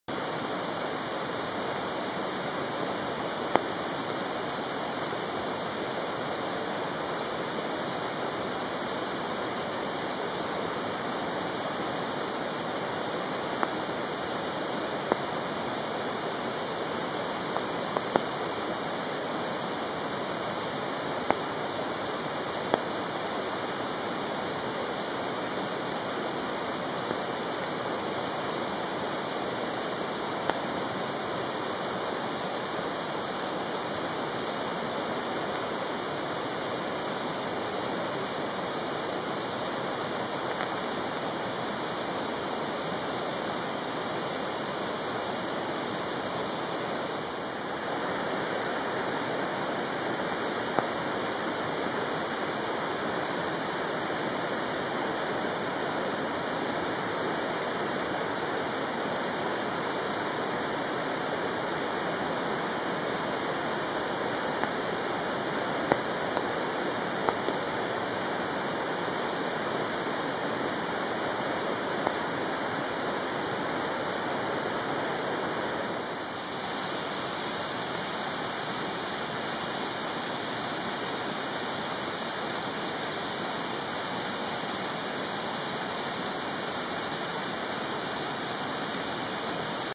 チャマダラエダシャク【蝶のような大型の蛾】
2021年9月上旬　長野県塩尻市の山地渓流沿い。チャマダラエダシャクという大型の蛾が、アザミの花に吸蜜に集まっていました。
まるでチョウのようで、斑紋の美しい昼行性のガです。